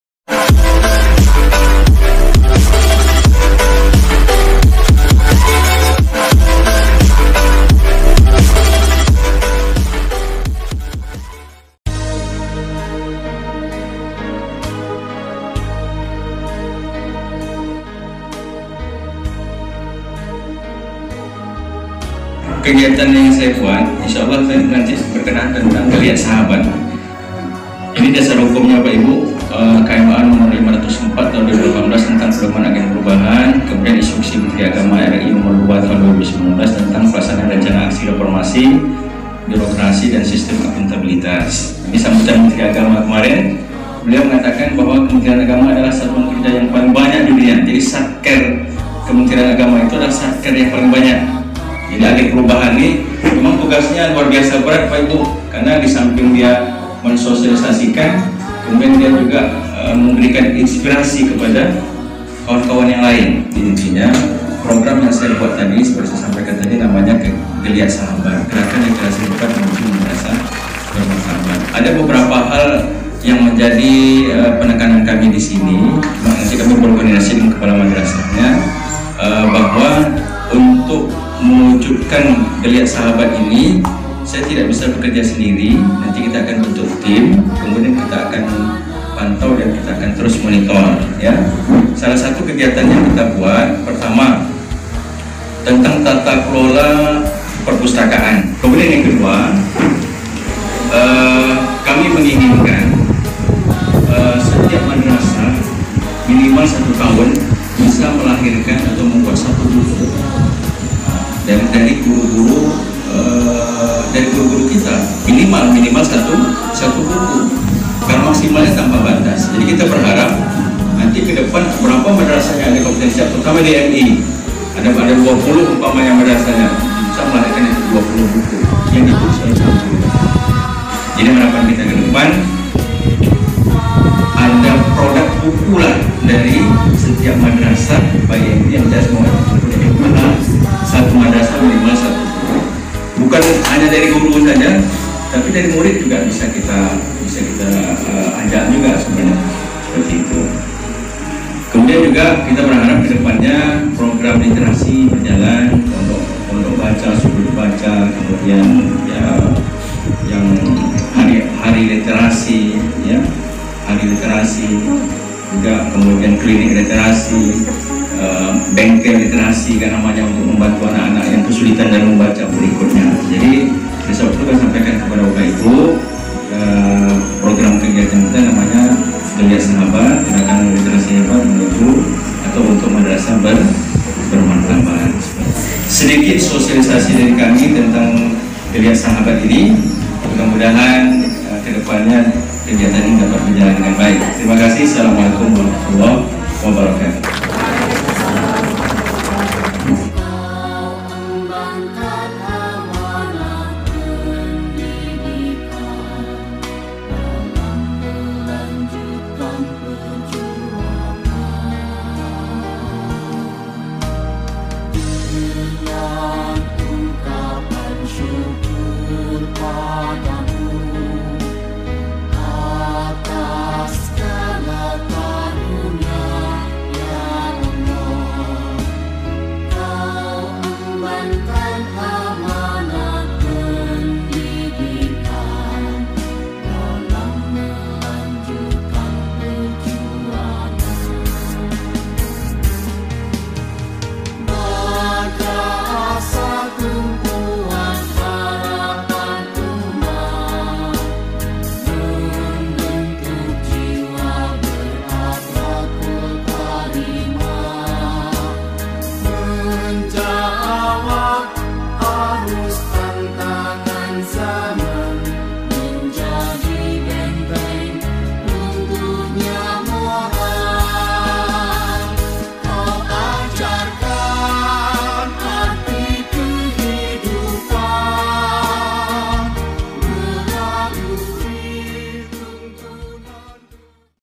Sosialisasi Agen Perubahan Kemenag Siak dan KKG MI Tanoto Foundation di MI As Sidiqiyah Siak
Assalamualaikum Wr Wb sahabat semua...

Kali ini Elanshory_Channel menayangkan video Sosialisasi Agen Perubahan Kemenag Kabupaten Siak dan KKG MI Mitra dan Non Mitra Tanoto Foundation di MI As Sidiqiyah Siak (31 Oktober 2019)

Bagi yang suka dengan video ini. Berikan masukan, saran yang membangun kepada kami untuk lebih meningkatkan lagi.

Mari kita bersama mencerdaskan anak bangsa... Terimakasih...